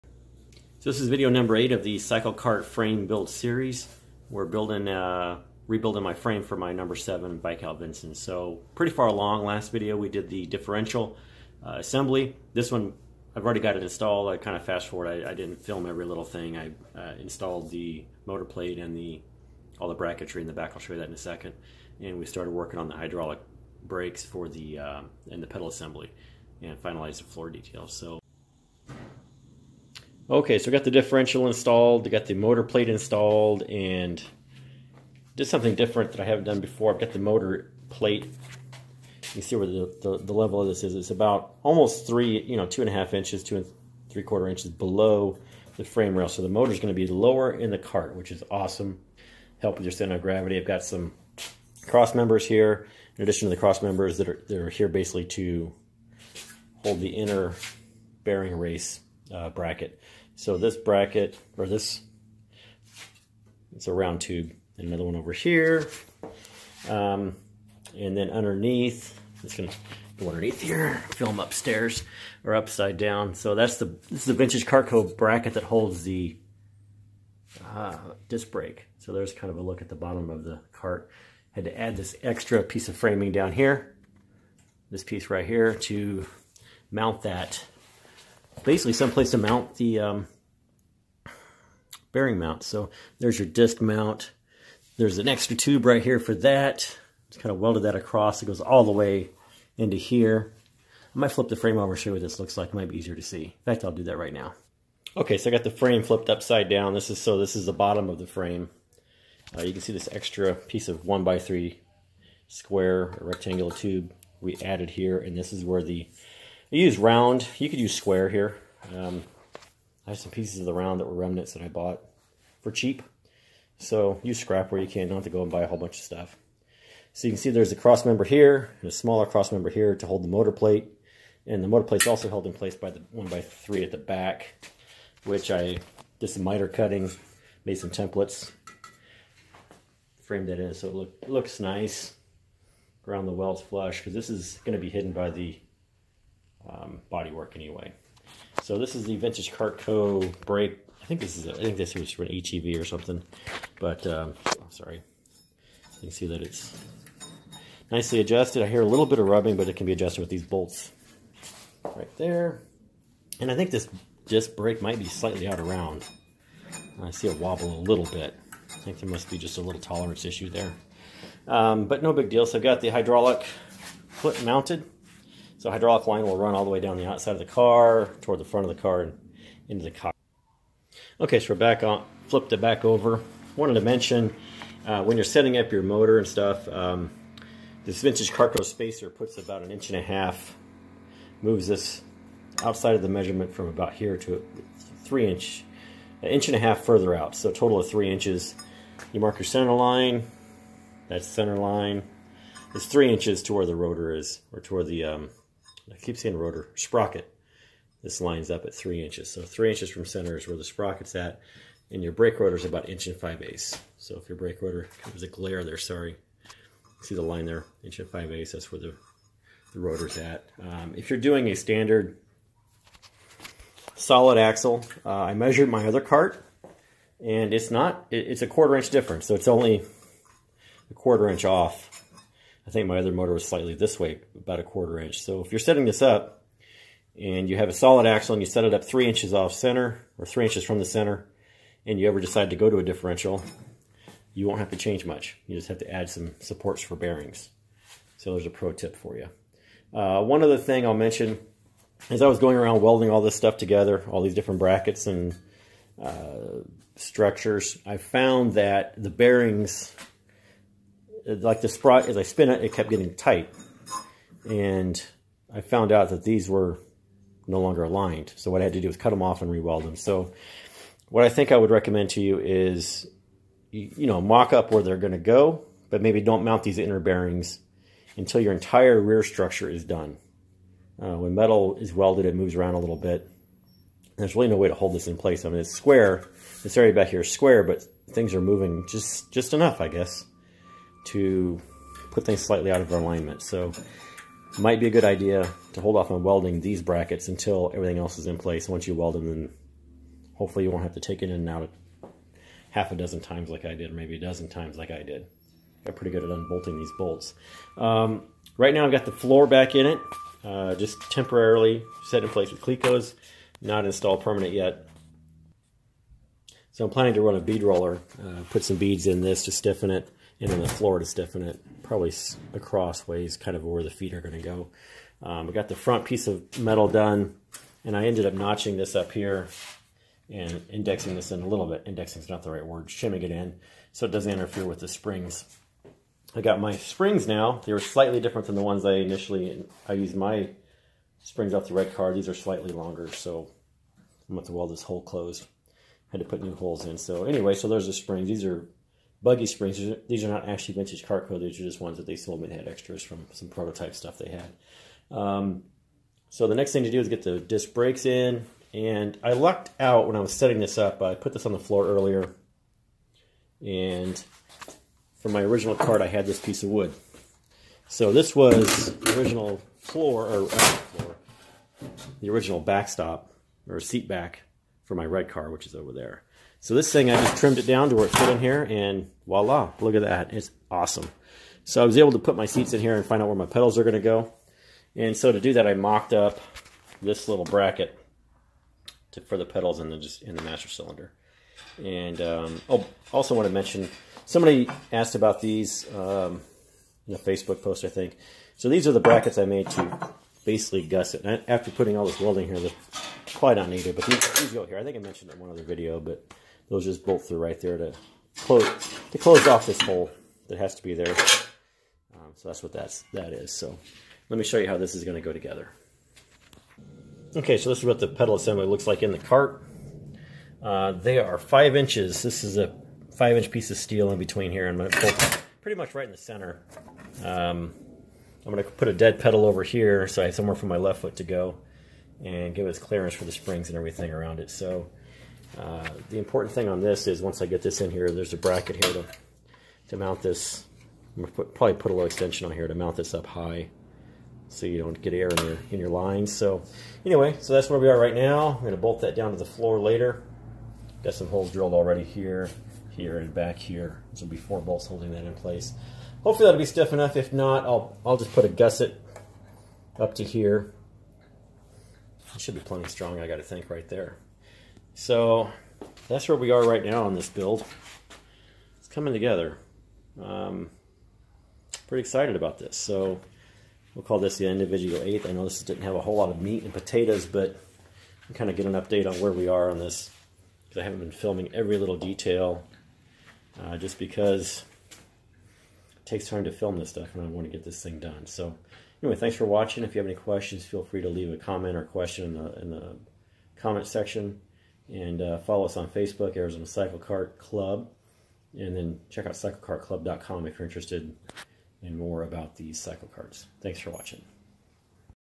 CycleKart Frame Video #8
Review of our progress. Completed differential installation and we start setting up the pedals.